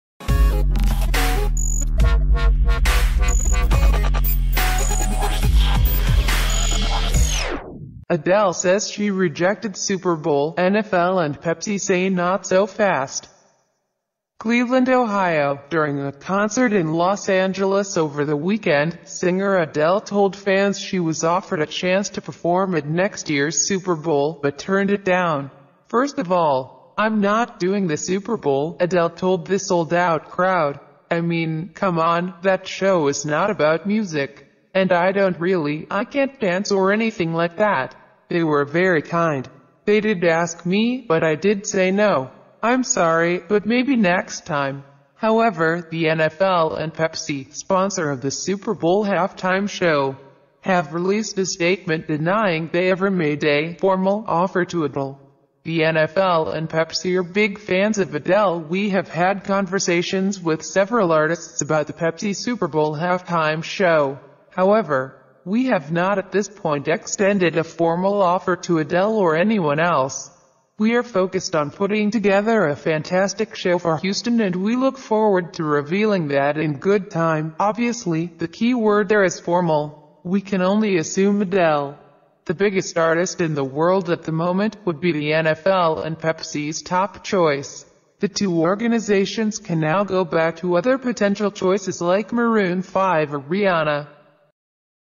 Adele says she rejected Super Bowl; NFL and Pepsi say not so fast
CLEVELAND, Ohio – During a concert in Los Angeles over the weekend, singer Adele told fans she was offered a chance to perform at next year's Super Bowl, but turned it down.

"First of all, I'm not doing the Super Bowl," Adele told the sold out crowd. "I mean, come on, that show is not about music. And I don't really...I can't dance or anything like that. They were very kind, they did ask me, but I did say no. I'm sorry, but maybe next time."

However, the NFL and Pepsi, sponsor of the Super Bowl halftime show, have released a statement denying they ever made a "formal" offer to Adele.

"The NFL and Pepsi are big fans of Adele We have had conversations with several artists about the Pepsi Super Bowl Halftime Show. However, we have not at this point extended a formal offer to Adele or anyone else. We are focused on putting together a fantastic show for Houston and we look forward to revealing that in good time."

Obviously, the key word there is formal. We can only assume Adele, the biggest artist in the world at the moment, would be the NFL and Pepsi's top choice. The two organizations can now go back to other potential choices like Maroon 5 or Rihanna.